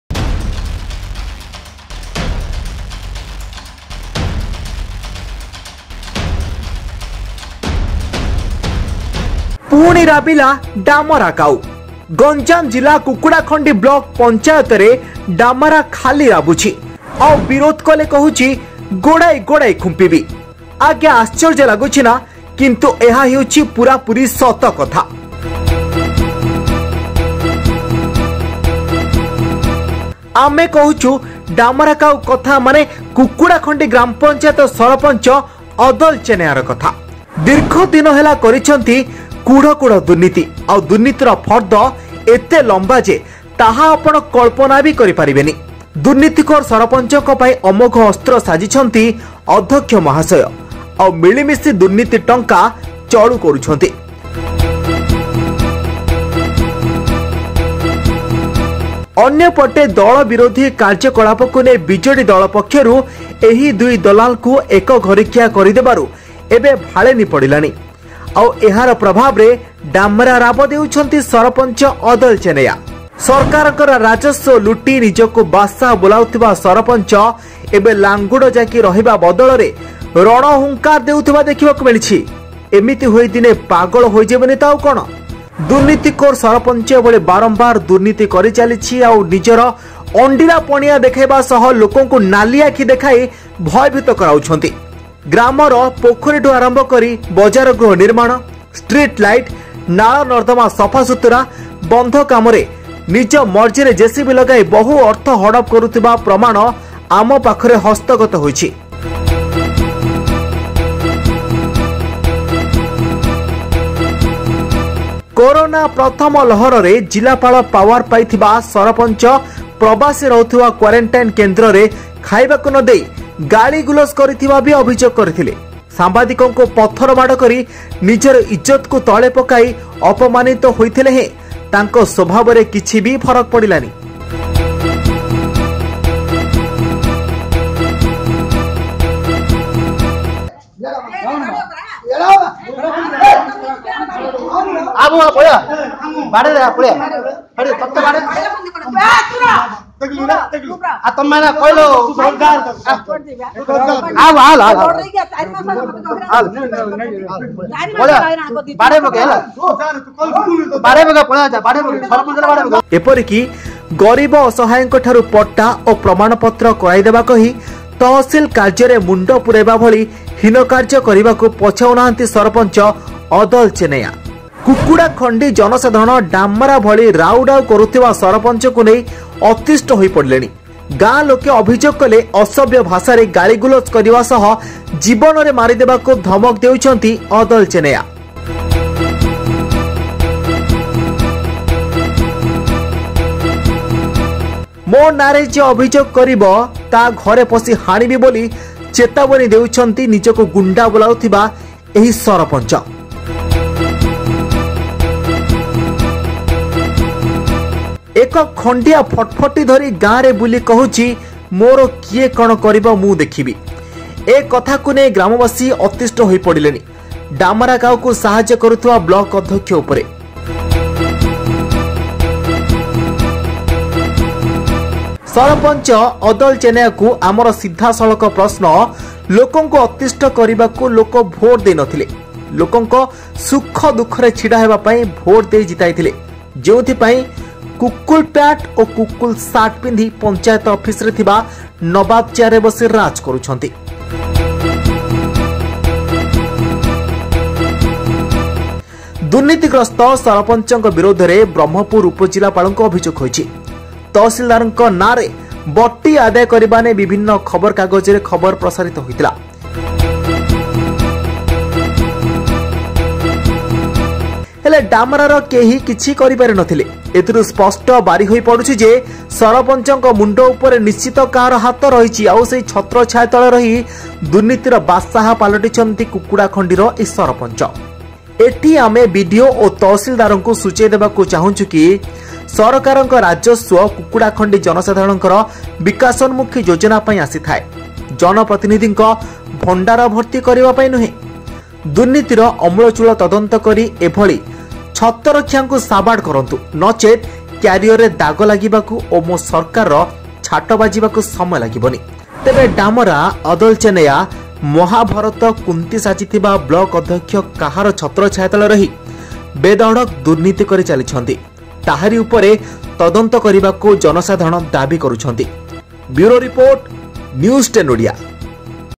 ଦୁର୍ନୀତିଖୋର ସରପଞ୍ଚର କଳା କାହାଣୀ || news10odia
କୁକୁଡ଼ାଖଣ୍ଡି ସରପଞ୍ଚ କୁଢ଼ କୁଢ଼ ଦୁର୍ନୀତି ଡାମରା କାଉ ଭଳି ରାବ ଦେଲେଣି  |